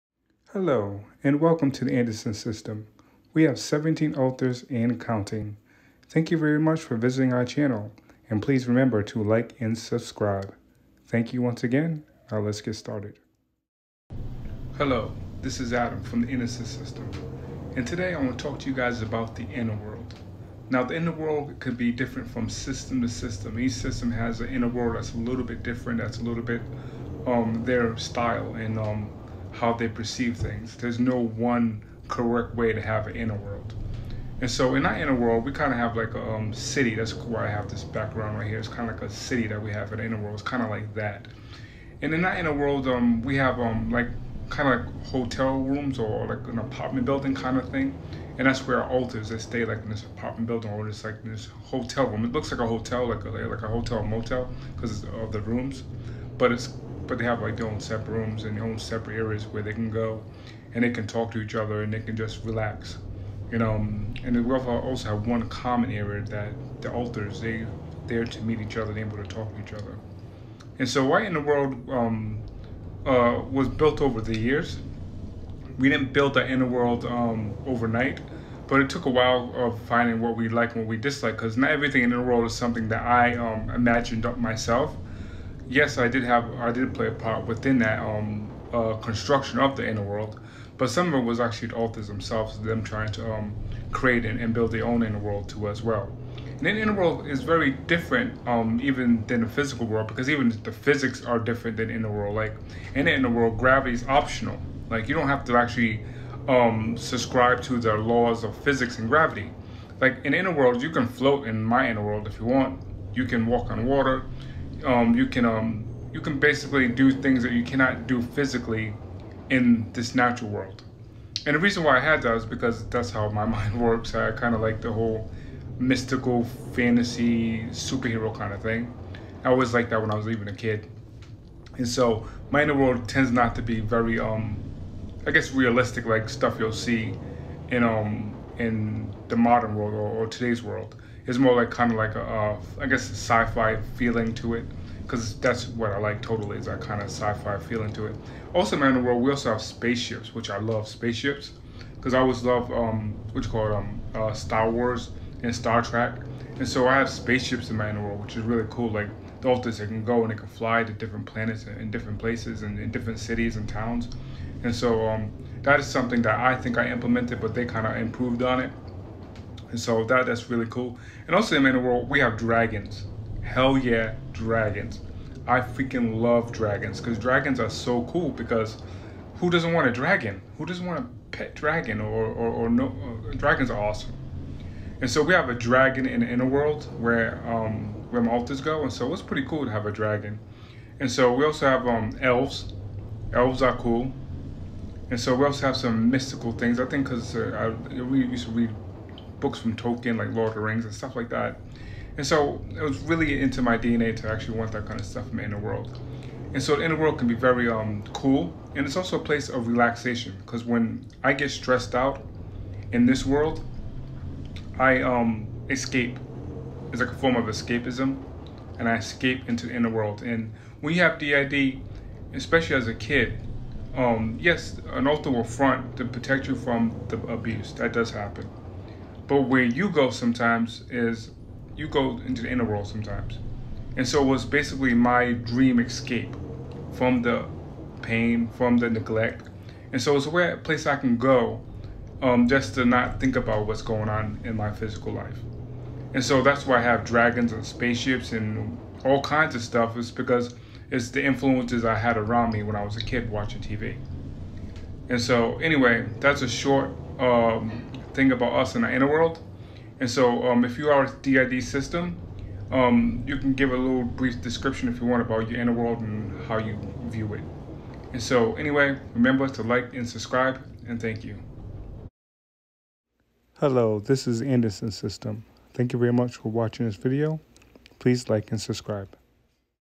Dissociative Identity Disorder and Our Inner World
This is a brief video of our inner world and how we see as a system with DID.